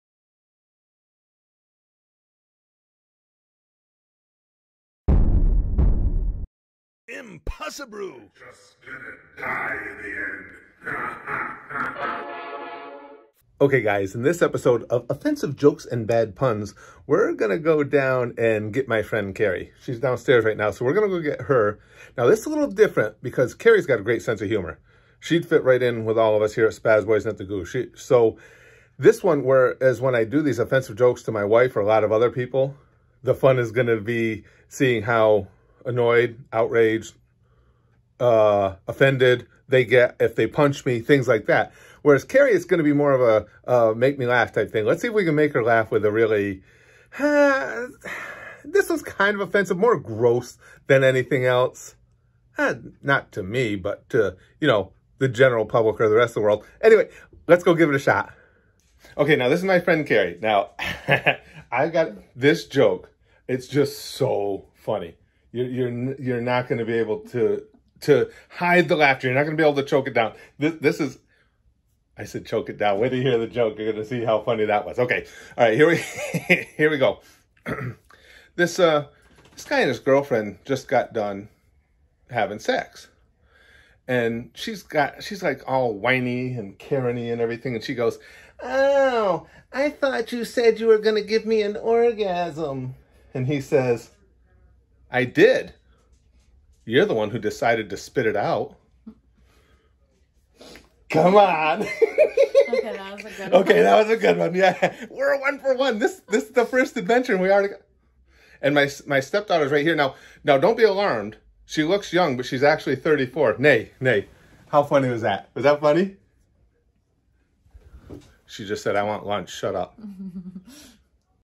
OFFENSIVE JOKES & BAD PUNS #1 - Can We Make Kari Laugh? 😆
Okay, in this series, we'll be seeing how well people take a joke or how they react to awful puns. This should be quite an adventure!... Our first victim is my friend, Kari. Let's see if she can resist the hilarity of this joke!   😁